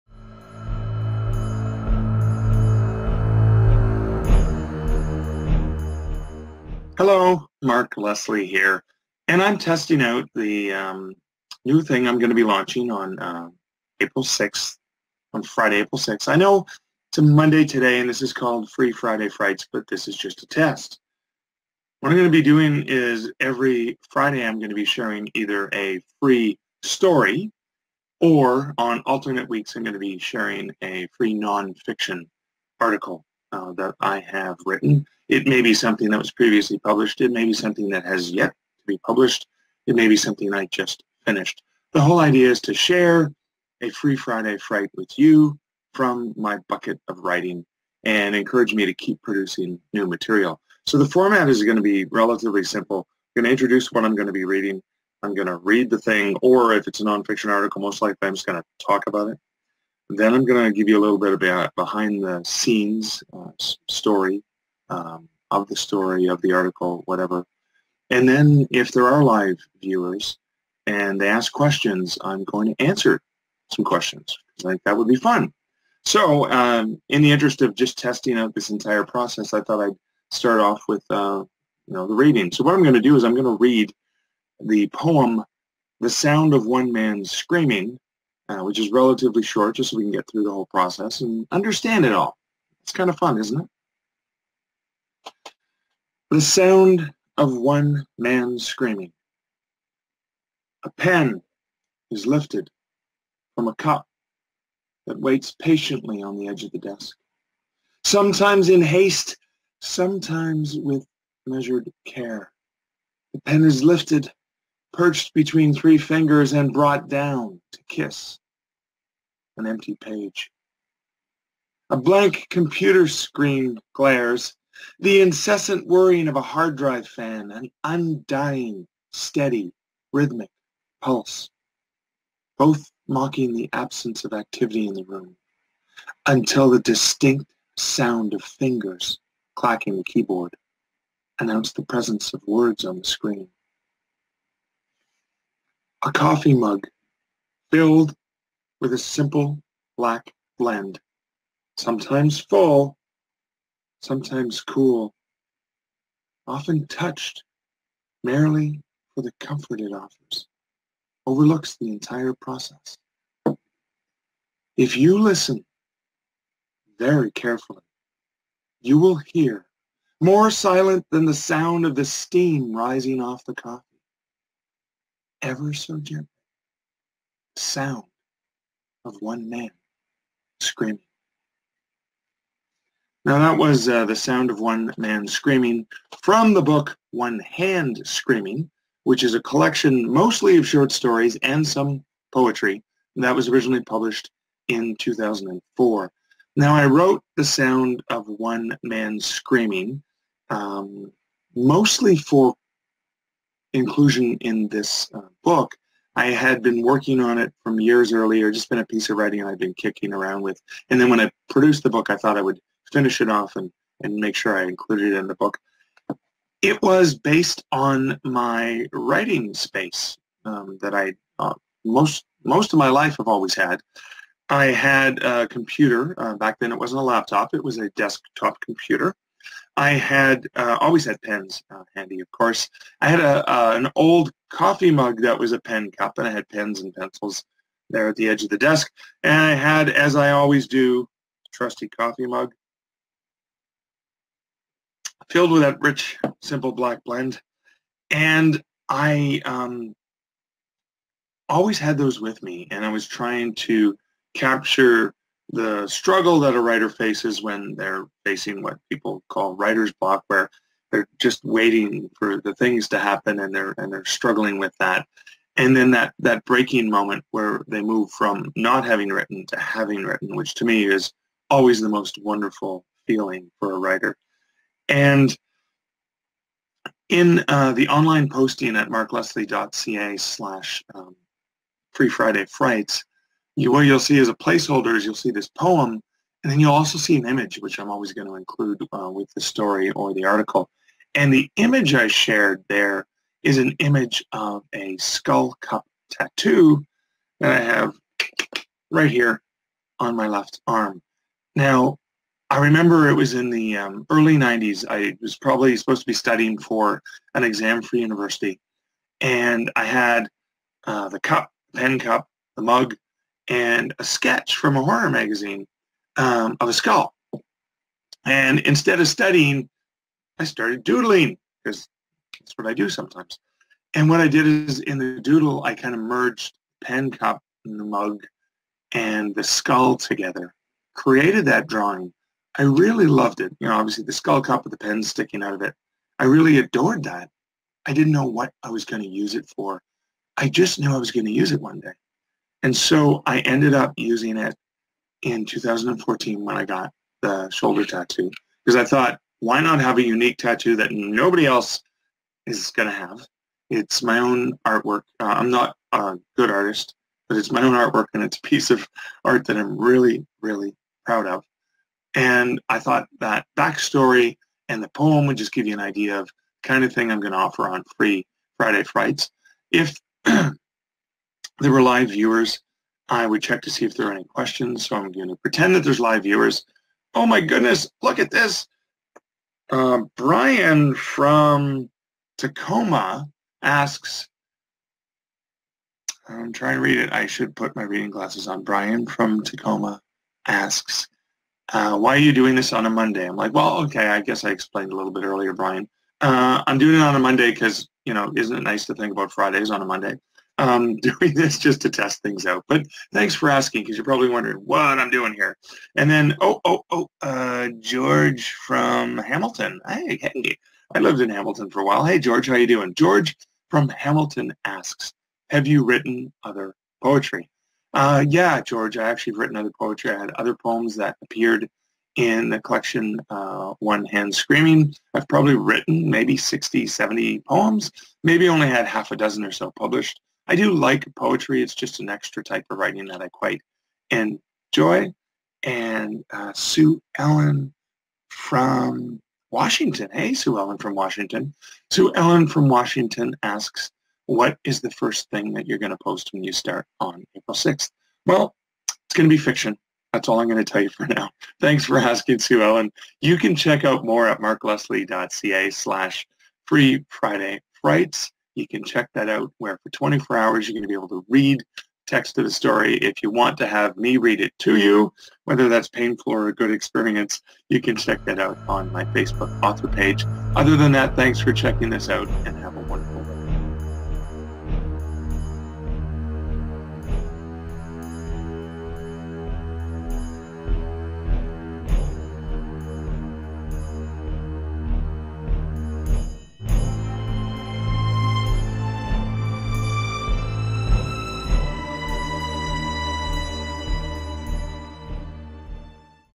Free Friday Frights - Introduction
FREE FRIDAY FRIGHTS - A weekly free read from Horror author Mark Leslie.

Alternating each week between fiction and non-fiction, an entire short story or short non-fiction true ghostly or eerie tale will be available to read for approximately 24 hours.

In additional, Mark will be doing a live reading of the fiction or a live discussion/storytelling of the non-fiction topic on his Facebook Author - that audio/video version will remain available beyond the Friday

The live video/reading will be structured in the following format: Brief Intro to that day's free fright, reading/relayed tale, a short behind-the-story/behind-the-research chat, followed by a Q&A.